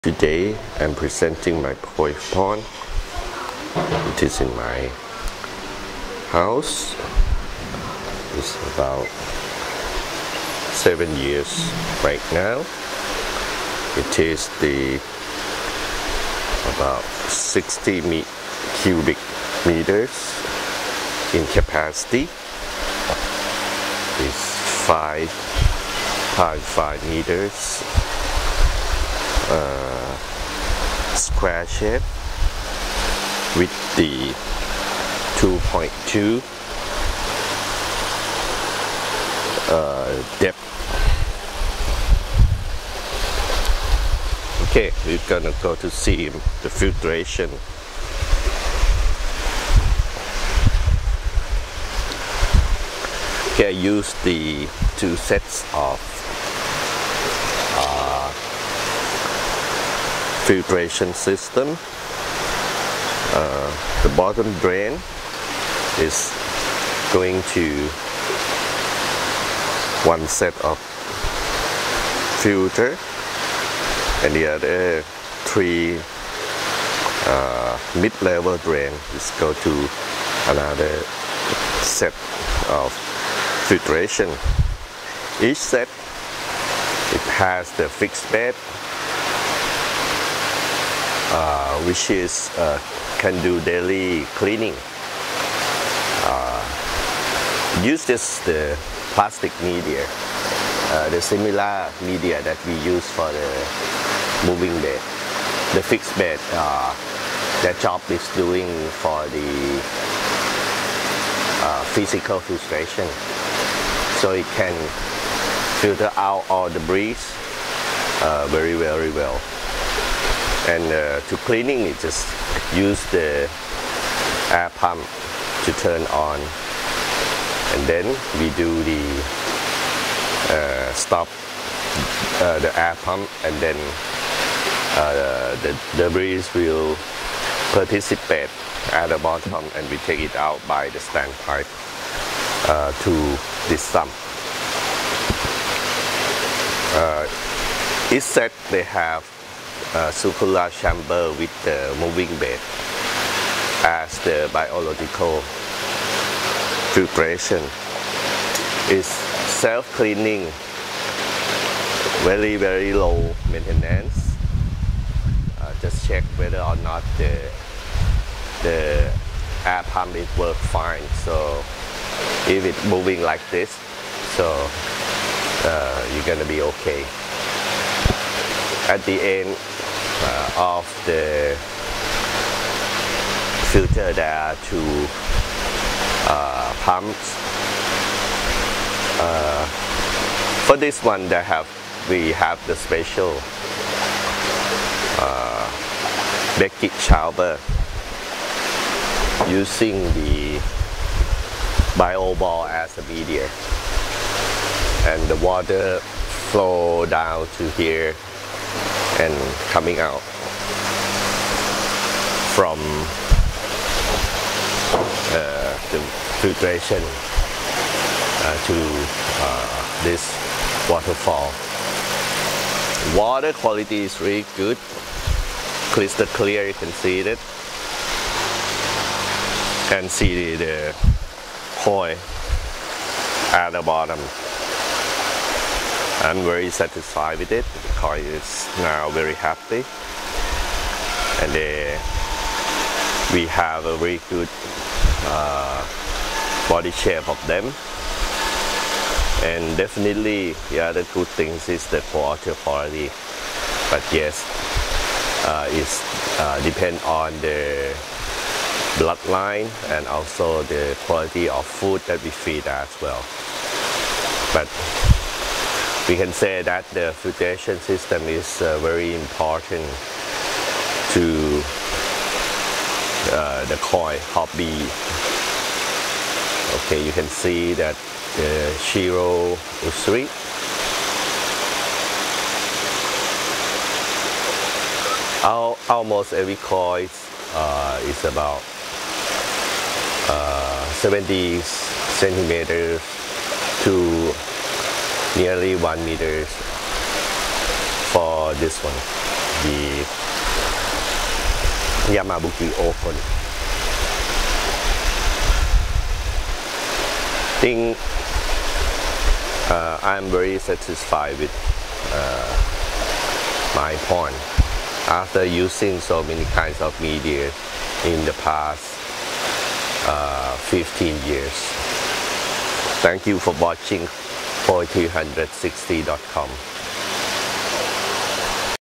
koi360.com: Moving bed and fixed bed filtration system
After 2 years of using this filtration, I am very satisfied with it.  I am using 2 sets of filtration system.  Each set has 1 fixed bed chamber and 4 circular moving bed chambers.  Fixed bed is used for physical filtration system which is cleaning everyday.  Moving bed is used for biological filtration which we take all debri out everyday by stand pipes to sump tank.  Fish are more healthy and eat more than before. Which help in body shape.  Definitely, very light effort for cleaning.
This pond designed by KlickAquatech.